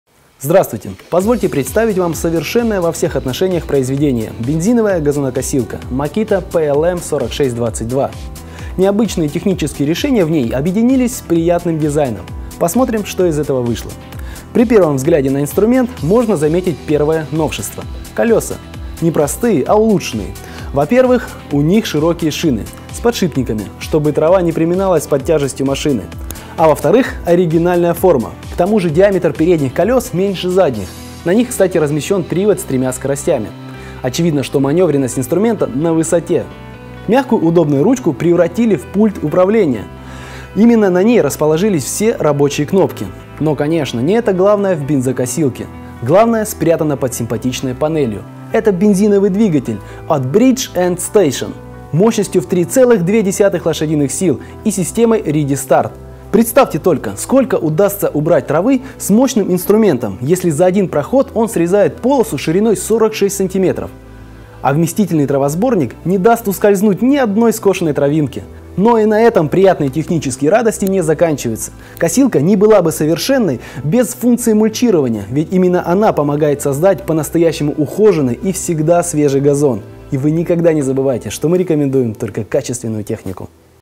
Обзор Газонокосилка MAKITA PLM4622 бензо
Самоходная трехскоростная модель MakitaPLM4622, с автоматизированным управлением и функцией мульчирования скошенной травы, стала одной из ярких новинок обновленного модельного ряда Makita.

Технические характеристики газонокосилки Makita PLM4622:

- Мощность агрегата в лошадиных силах составляет: от 3.2 л. до 3.5 л.с.
- Мощность потребления: 2540 Вт
- Рабочий объем: 190см3
- Ширина захвата травы при скашивании: 460 мм
- Три скорости передвижения газонокосилки: 3.0/3.4/4.5 км/ч
- Пять положений для градации толщины среза при скашивании --от 20мм до 75 мм
- Объем травосборника: 60 л.
- Вес: 35 кг
- Площадь газона: 1800 м2
- Тип двигателя 675 EX
- Безаккумуляторный ручной стартер
- Автоматический акселератор газа
- 4 Сut - технология скашивания
- Удобная установка травосборника
- В комплект входит заглушка для мульчирования
- Колеса снабжены шариковыми подшипниками
- Панель автоматизированного управления, для удобства выведена на ручку инструмента